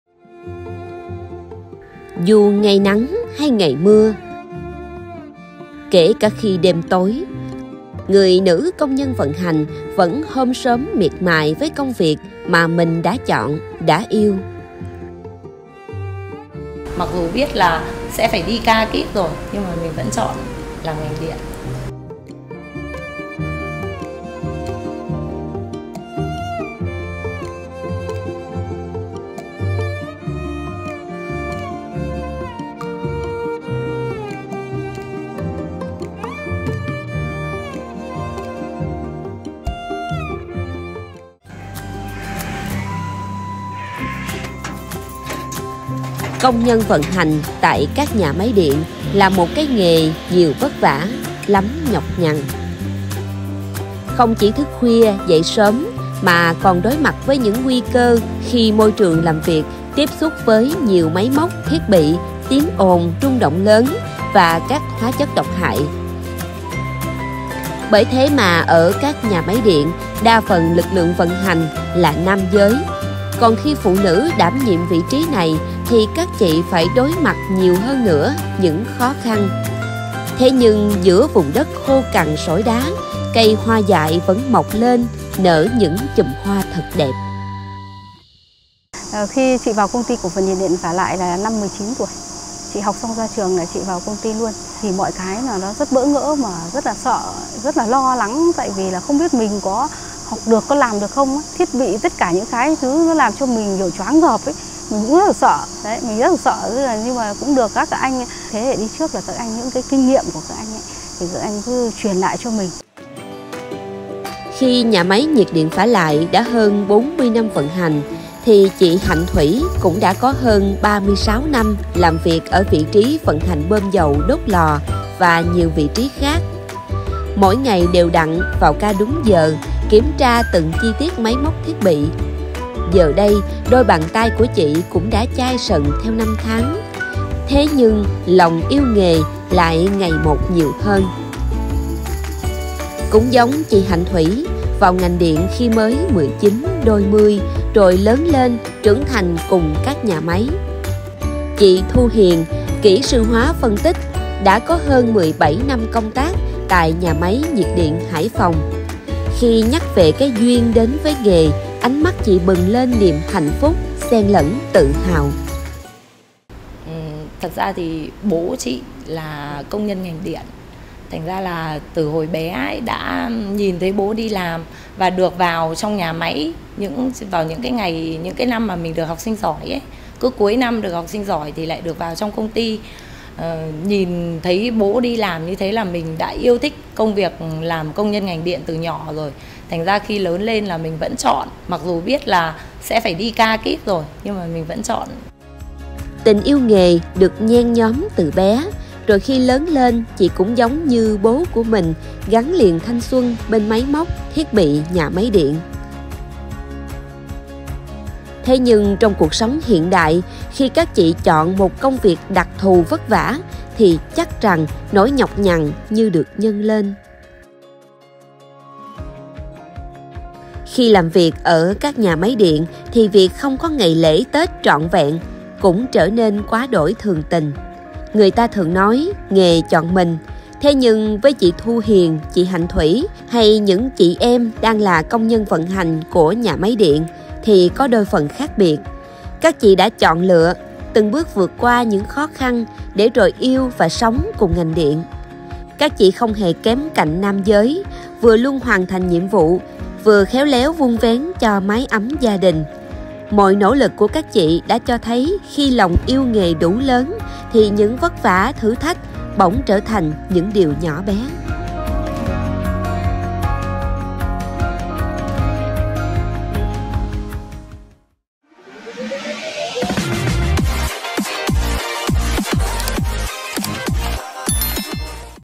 KHI PHỤ NỮ LÀ CÔNG NHÂN NGÀNH ĐIỆN
Những điều “đặc biệt” chỉ có ở phụ nữ ngành
Điện.
© Bản quyền video thuộc Tập đoàn Điện lực Việt Nam
#20/10